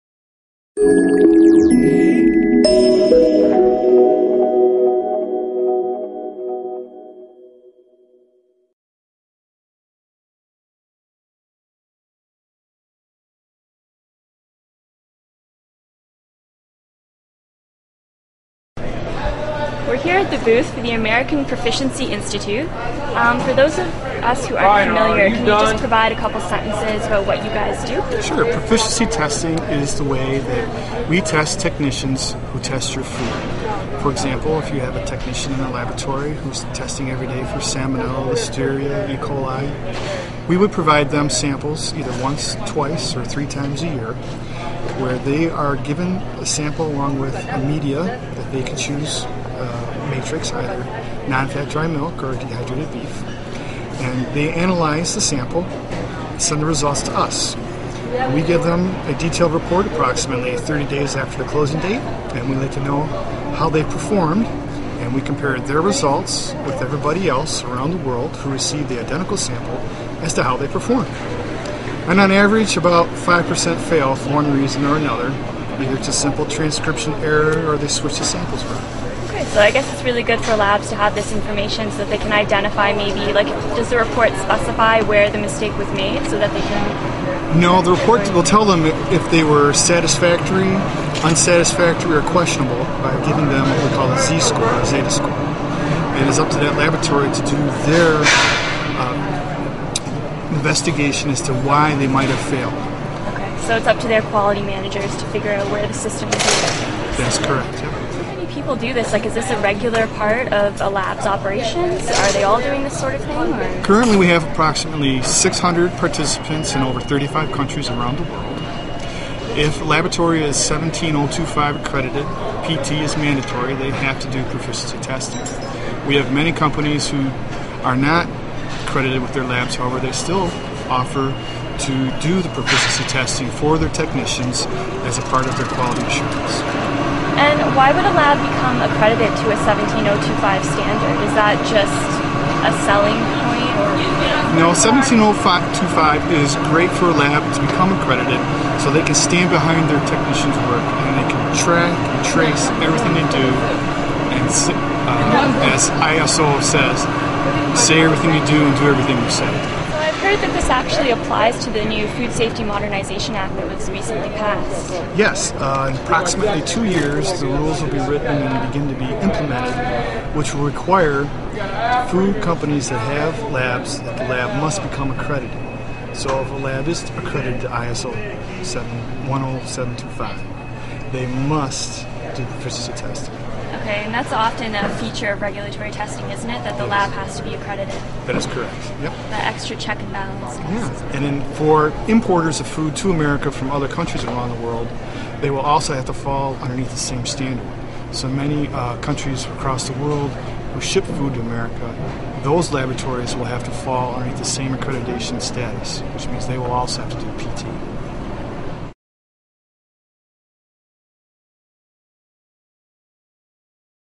Christopher Snabes, Food Technical Specialist with the American Proficiency Institute
Christopher Snabes, Food Technical Specialist with the American Proficiency Institute, talks with Global Food Safety Resource New Media Editor, Allie Gallant about new regulatory requirements stemming from the Food Safety Modernization Act regarding QA management and accreditation of food testing laboratories to the ISO 17025 standard, as well as QA support provided by API's proficiency testing.